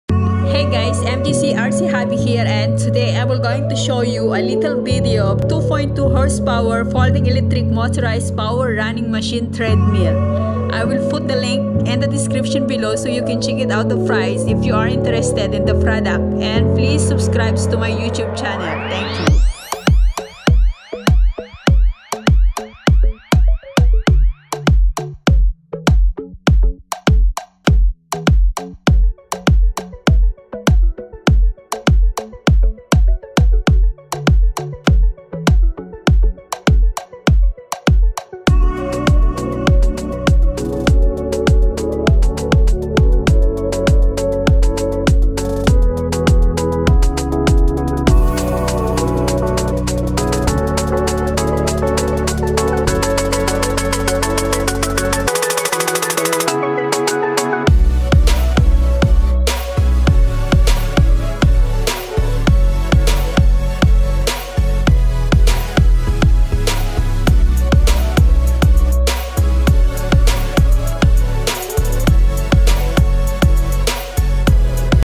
2.25 HP Folding Electric Motorized Power Running Machine/Best Under Desk WalkingPad Treadmill 2020
If you living area does not much space for fitness equipment,here the best slimflat holding treadmill that you can store under your bed or sofa.
 Build your home gym...
It's a perfect complement to your room and office.

2.25 HP Impulse Max Motor & Weight Capacity.
Foldable & Adjustable Incline Design.
0.5-7.5 MPH Speed levels & P01-P12 Programs.
5" Blue-Ray LCD Display & 16" x 47" Rubber Tread Belt.
Safety Design and Cushioning Design

You can check out the link below for the price or read some extra reviews from customer to find out more base from their experience.

2.25 HP Folding Electric Motorized Power Running Machine